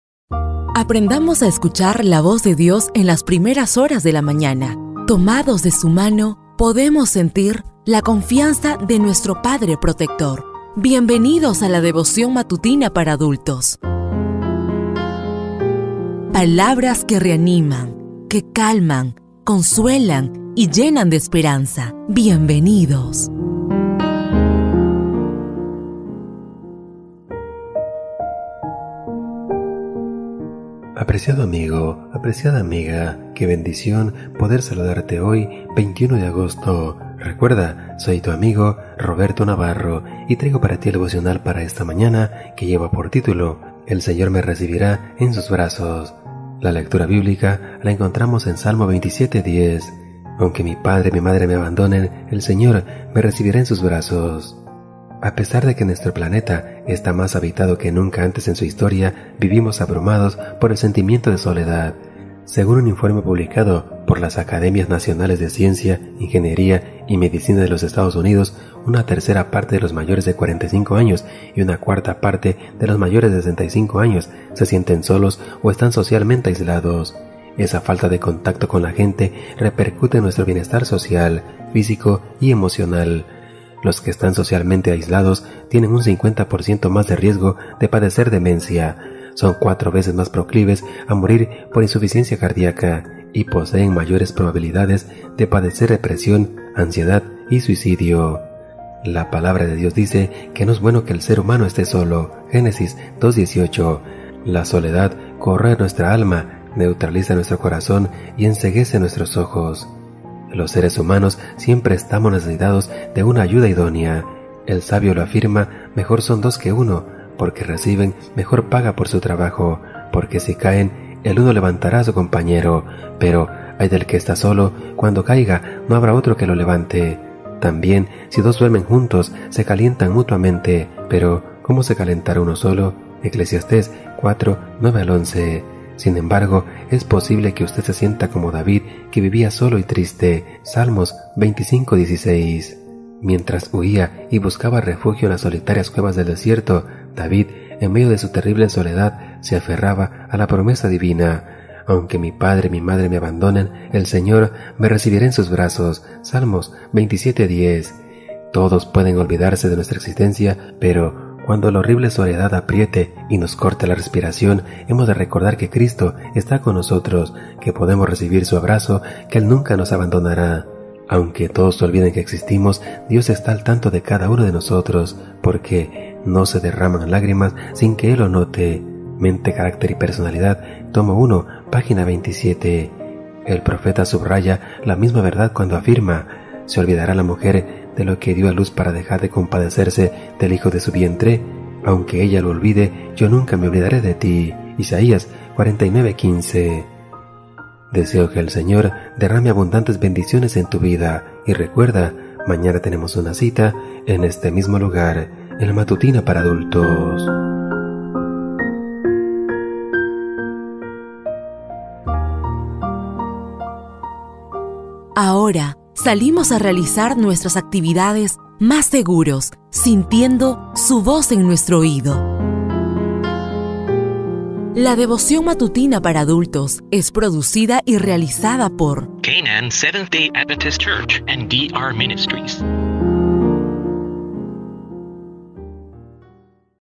Devoción Matutina Para Adultos - 21 de Agosto del 2023
21 DE AGOSTO
"EL SEÑOR ME RECIBIRÁ EN SUS BRAZOS"

Aunque mi padre y mi madre me abandonen, el Señor me recibirá en sus brazos (Salmo 27:10, NVI).

A pesar de que nuestro planeta está más habitado que nunca antes en su historia, vivimos abrumados por el sentimiento de soledad. Según un informe publicado por las Academias Nacionales de Ciencias, Ingeniería y Medicina de los Estados Unidos, una tercera parte de los mayores de 45 años, y una cuarta parte de los mayores de 65 años se sienten solos o están socialmente aislados. Esa falta de contacto con la gente repercute en nuestro bienestar social, físico y emocional. Los que están socialmente aislados tienen un 50 por ciento más de riesgo de padecer demencia; son cuatro veces más proclives a morir por insuficiencia cardiaca; y poseen mayores probabilidades de padecer depresión, ansiedad y suicidio. *
La Palabra de Dios dice que no es bueno que el ser humano "esté solo" (Génesis 2:18). La soledad corroe nuestra alma, neutraliza nuestro corazón y enceguece nuestros ojos. Los seres humanos siempre estamos necesitados de una "ayuda idónea". El Sabio lo afirma: "Mejor son dos que uno, pues reciben mejor paga por su trabajo. Porque si caen, el uno levantará a su compañero; pero ¡ay del que está solo! Cuando caiga no habrá otro que lo levante. También, si dos duermen juntos, se calientan mutuamente, pero ¿cómo se calentará uno solo?" (Eclesiastés 4:9-11). Sin embargo, es posible que usted se sienta como David, que viva "solo y triste" (Salmo 25:16, PDT).
Mientras huía y buscaba refugio en las solitarias cuevas del desierto, David, en medio de su terrible soledad, se aferraba a la promesa divina: "Aunque mi padre y mi madre me abandonen, el Señor me recibirá en sus brazos" (Salmo 27:10, NVI). Todos pueden olvidarse de nuestra existencia, pero cuando la horrible soledad apriete y nos corte la respiración, hemos de recordar que Cristo está con nosotros, que podemos recibir su abrazo, que él nunca nos abandonará. Aunque todos olviden que existimos, Dios está al tanto de cada uno de nosotros, porque "no se derraman lágrimas sin que él lo note" (Mente, carácter y personalidad, tomo 1, p. 27). El profeta subraya la misma verdad cuando afirma: "¿Se olvidará la mujer de lo que dio a luz, para dejar de compadecerse del hijo de su vientre? ¡Aunque ella lo olvide, yo nunca me olvidaré de ti!" (Isaías 49:15).
* National Academies of Sciences, Engineering, and Medicine, Social Isolation and Loneliness in Older Adults: Opportunities for the Health Care System (Washington, D. C.: The National Academies Press, 2020).